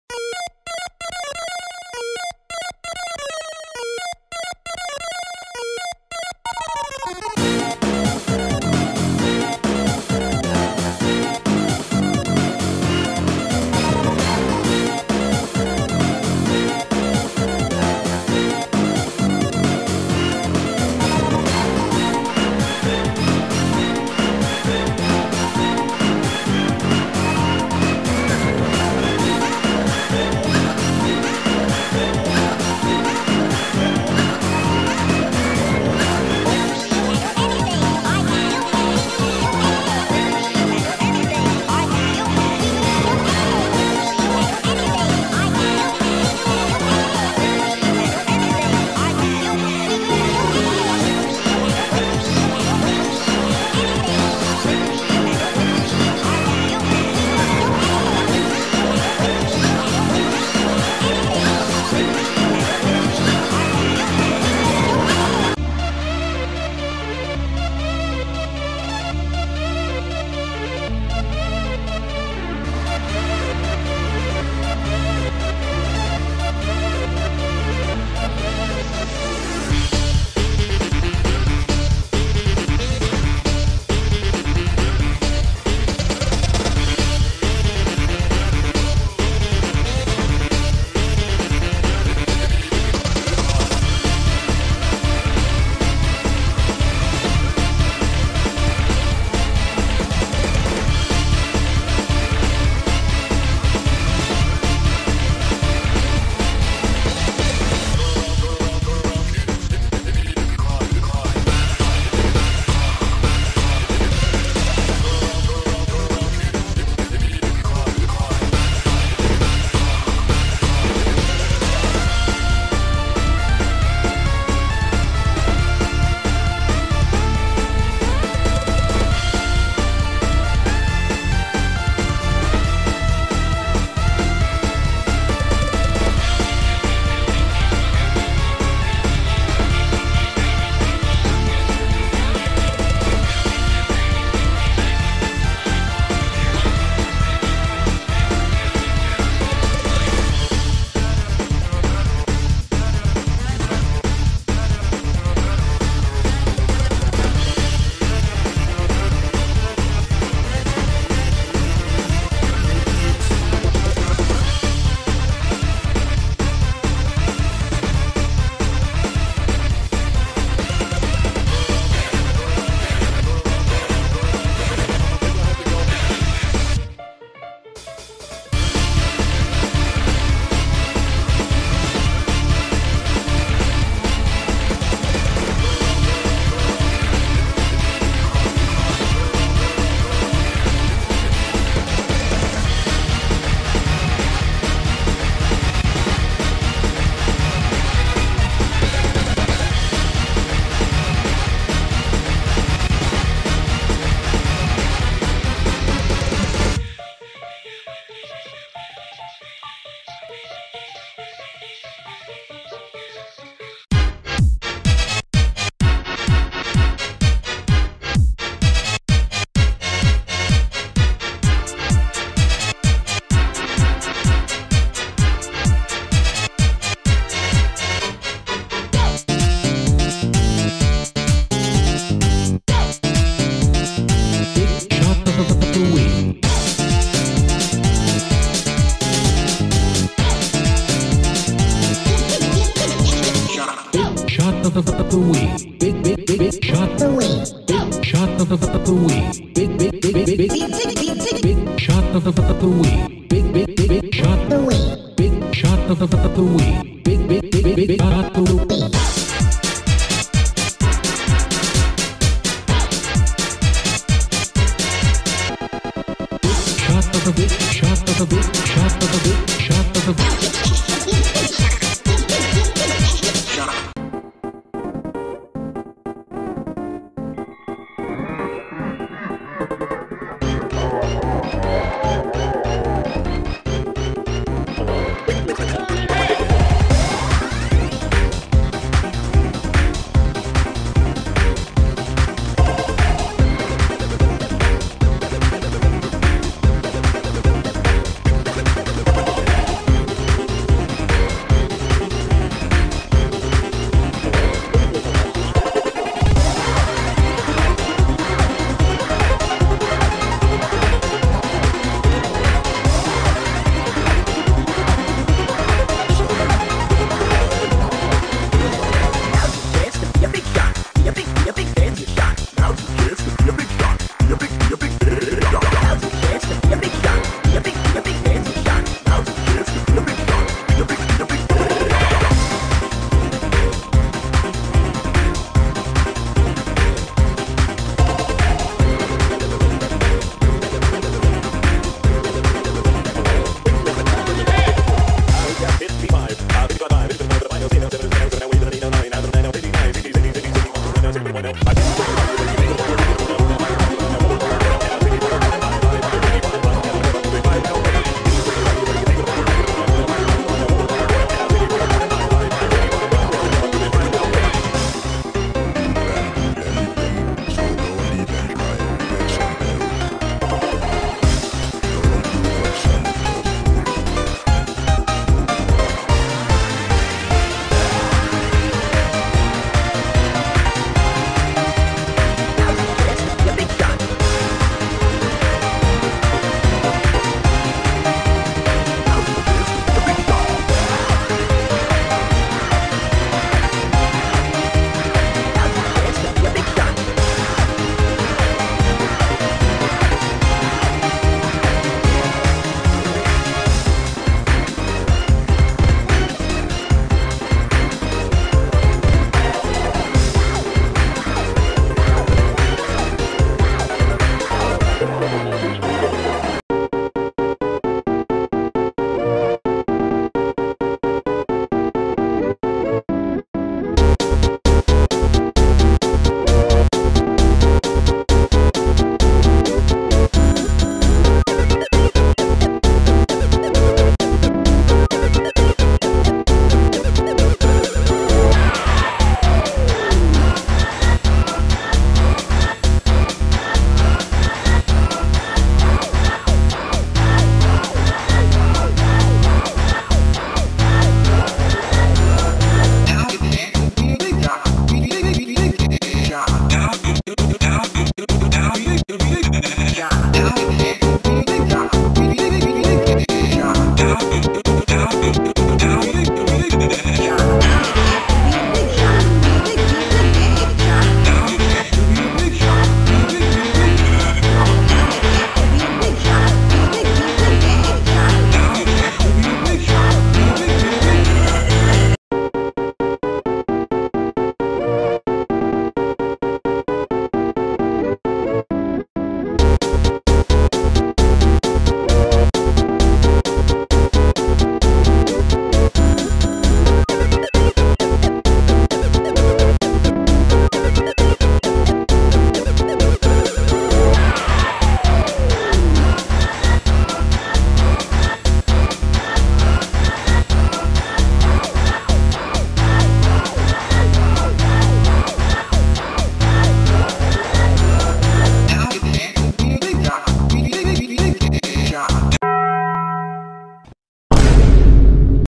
Drawing N.Igmaton NEO (Spamton + N.Igma) [Krita]
Original art by S.N. (me)
N.Igmaton NEO by S.N.
N.Igma by JenL
Spamton by Toby Fox